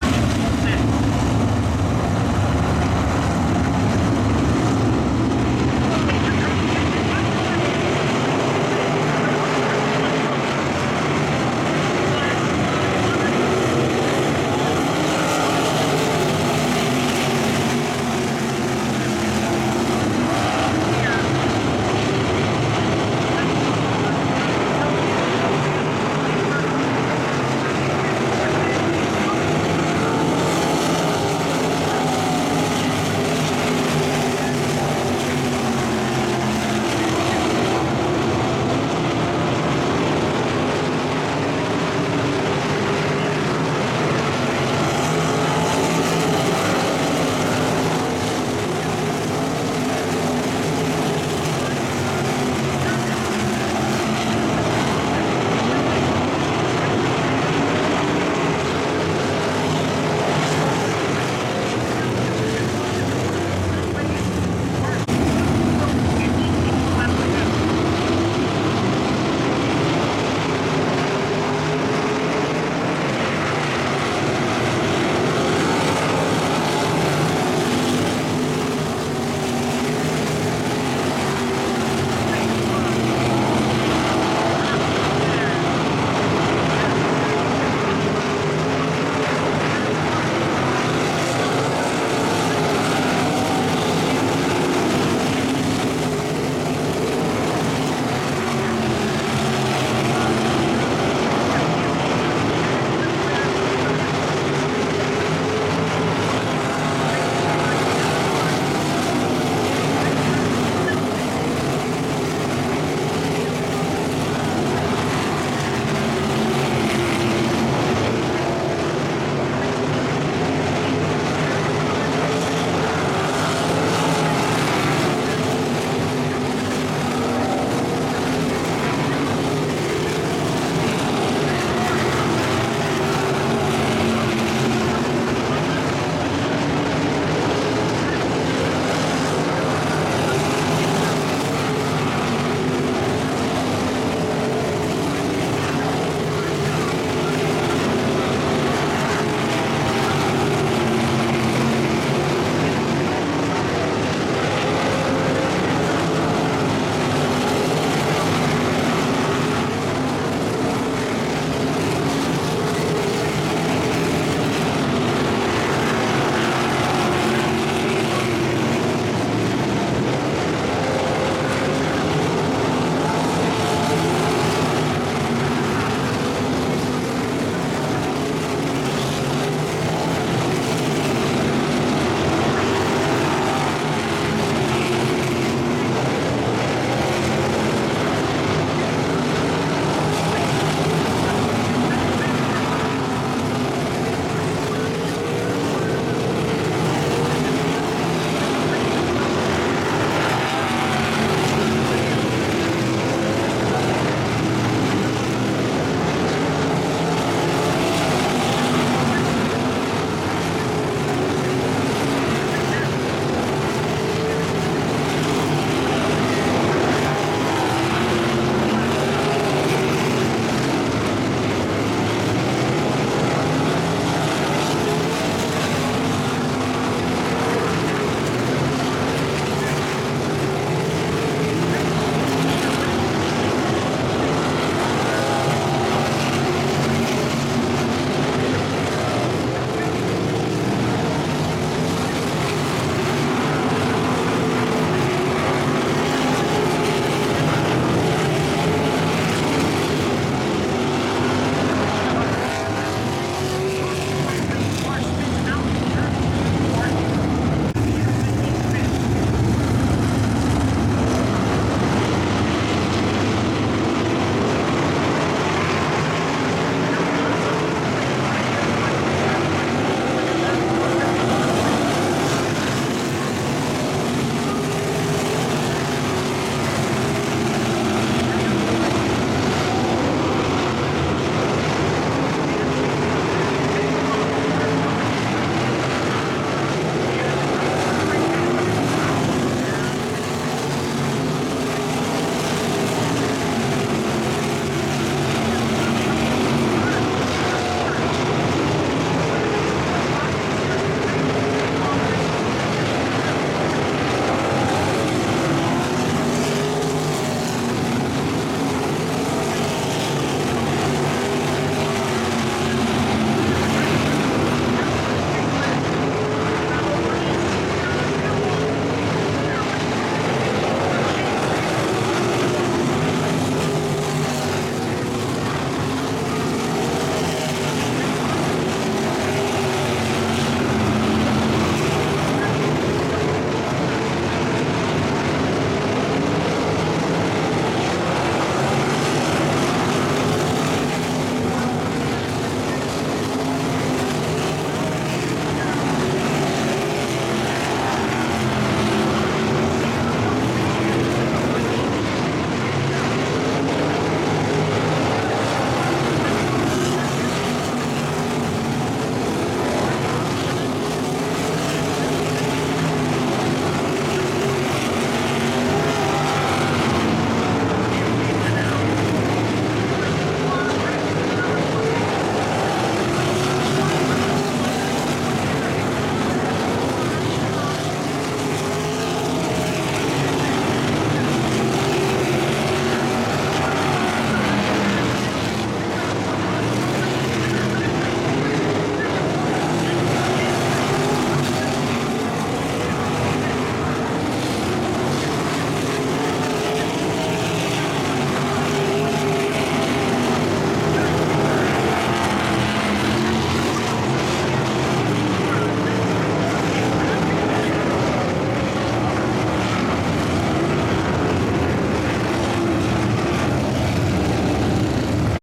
UMSS Traditional Sprint Feature - Cedar Lake Speedway 07/27/2024
Heat race winners Blake Anderson and Cam Schafer swap the lead in the closing laps of the UMSS Traditional Sprint feature at Cedar Lake Speedway on 07/27/2024